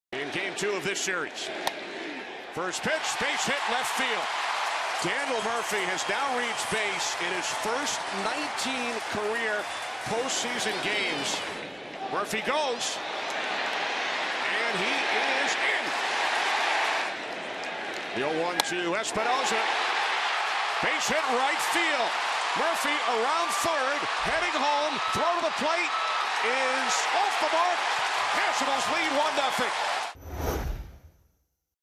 LAD@WSH Gm5: Murphy manufactures a run in the 2nd
Daniel Murphy's hitting and baserunning helps push across the Nationals' first run, giving them a 1-0 lead in the bottom of the 2nd